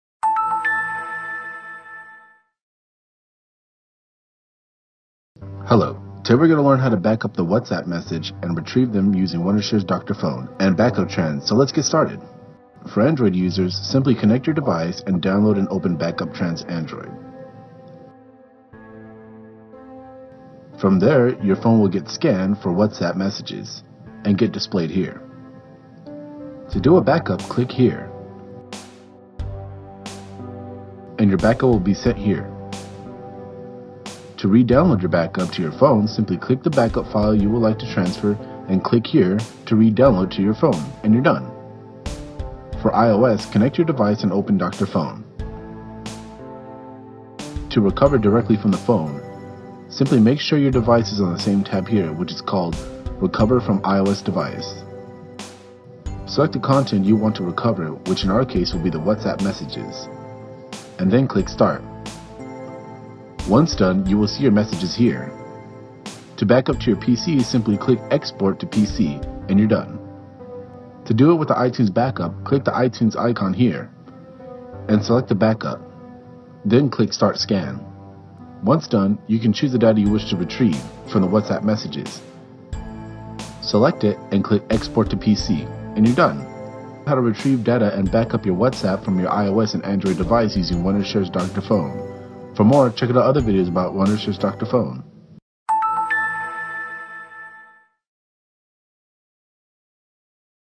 WhatsApp Backup Extractor: Read WhatsApp Conversations on your Computer
If you have ever wanted to read your WhatsApp messages on your computer, you should be happy to know that you can simply extract your WhatsApp backup to do so. It may be that you want to look at old archived messages that may have important information that you want to refer to. It could also be that you simply want to see the messages you sent and received a while back. Whatever the reason, extracting your WhatsApp backup can allow you to do this and in this video we are going to be looking at how to do that effectively.